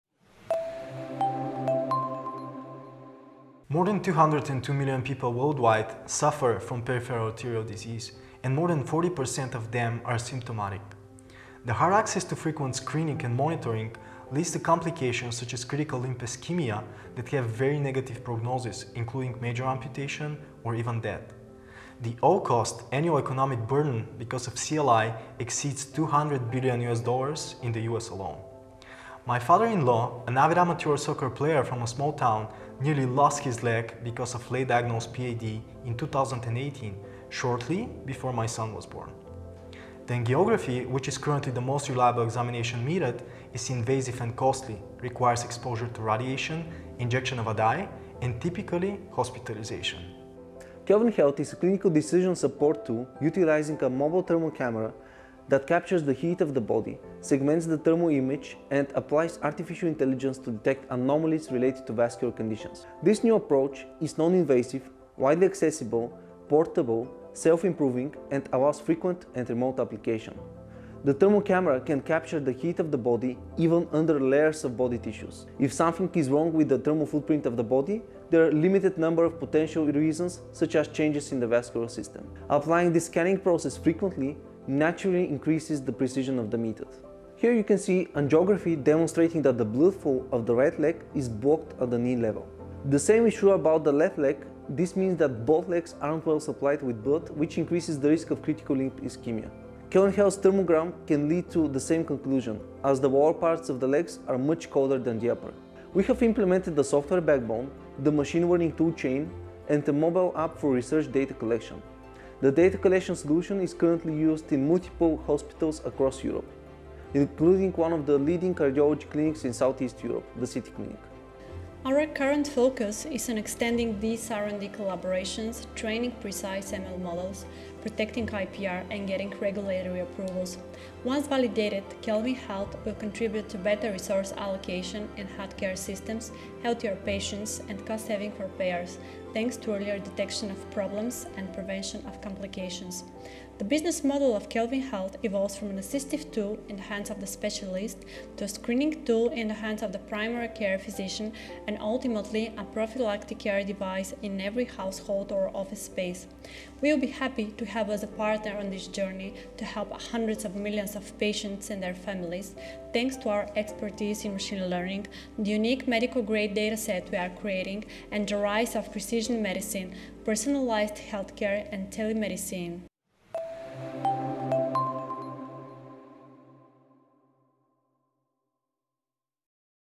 Screening, monitoring, and early diagnostics of PAD and CLI - Kelvin Health
Mobile thermography powered by AI automated image processing for screening and monitoring of health-related conditions directly linked to blood vessels, and respectively body thermodynamics is currently only covered in scientific literature. Various studies from around the world (mostly in the US, Israel & India) prove the applicability of thermography for detecting breast cancer, fatty liver disease, cardiac remodeling, and even Covid19, but none has led to a product of a great societal and commercial value yet.  Kelvin is to be the first medical-grade AI thermography product able to detect structural and functional changes in the cardiovascular system, and specifically the so-called Peripheral Arterial Disease (PAD).